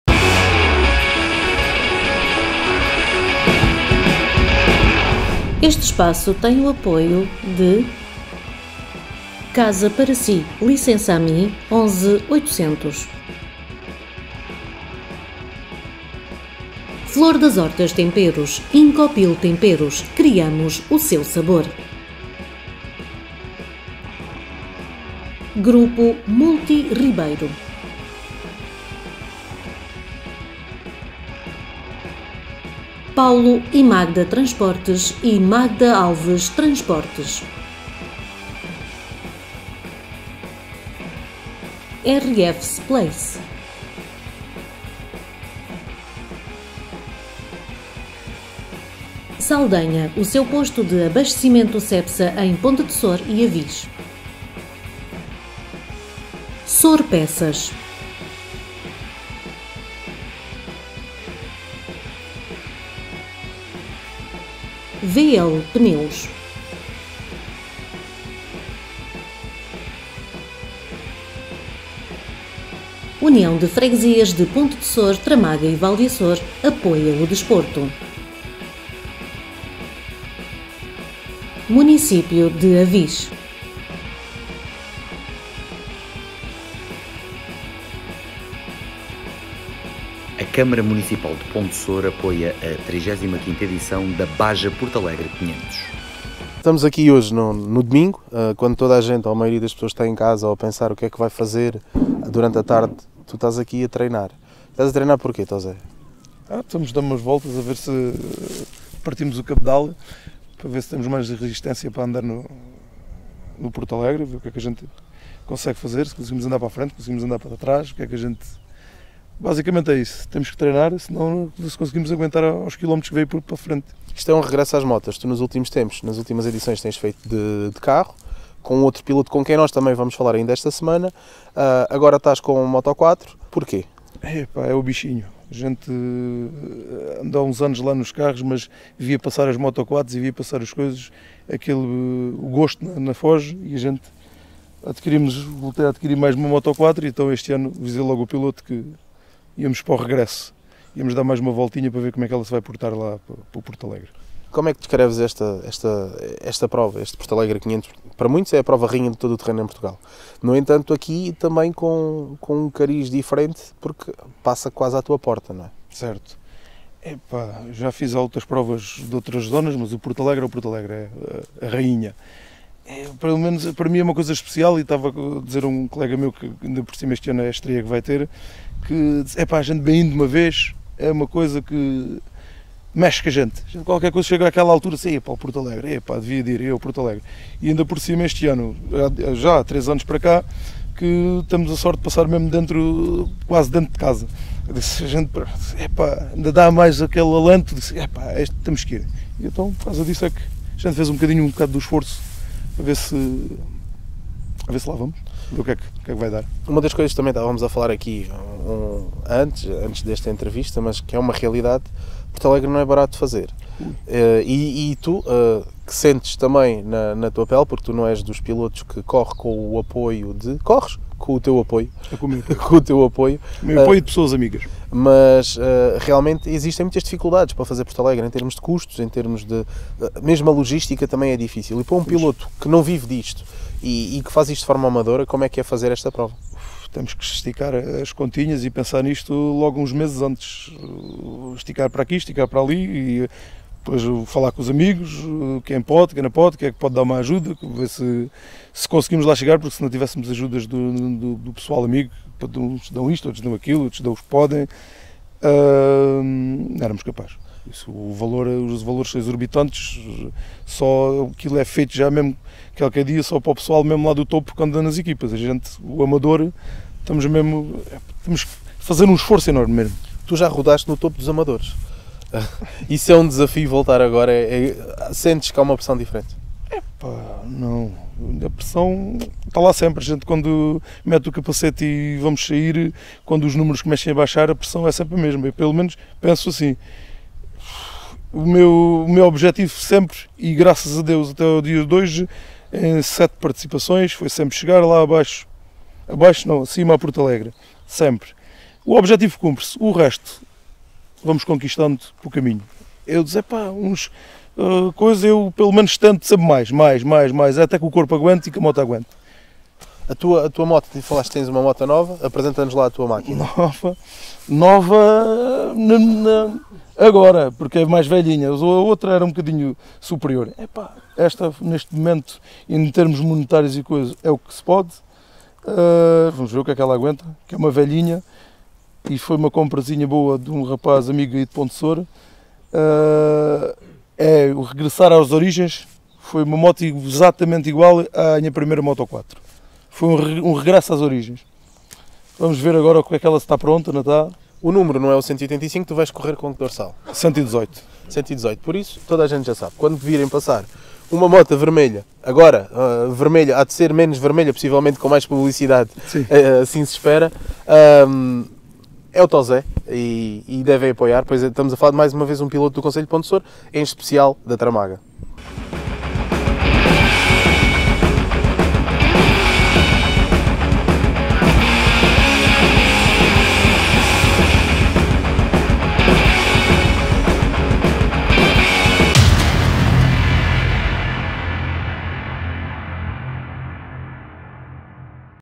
O BICHINHO DAS MOTO-QUATRO FEZ-ME VOLTAR
Entrevista ao piloto Antonio Veigas (Vitalino) a falar sobre a participação na 35ª Baja portalegre 500, com regresso às moto-quatro.

PRODUÇÃO:
Digital Layer Produção AudioVisual